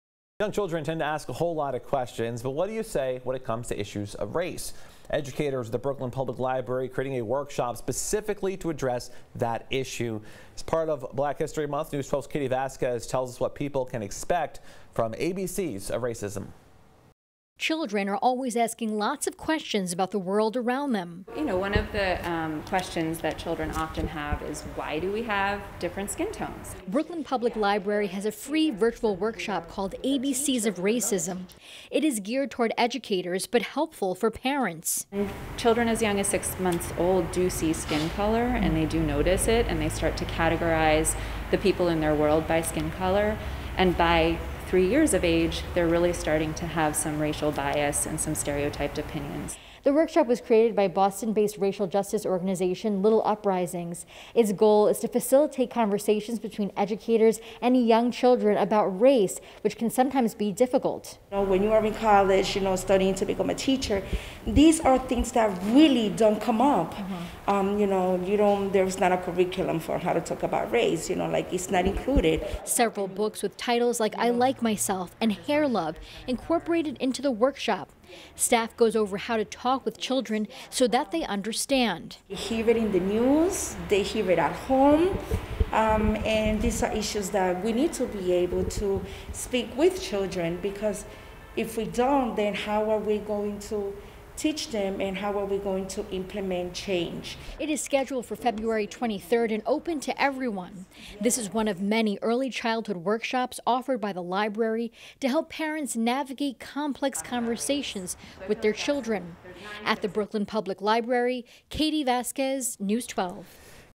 Brooklyn Public Library offers workshop on how to talk to children about race
Educators at the Brooklyn Public Library have created a workshop on how to talk to children about race.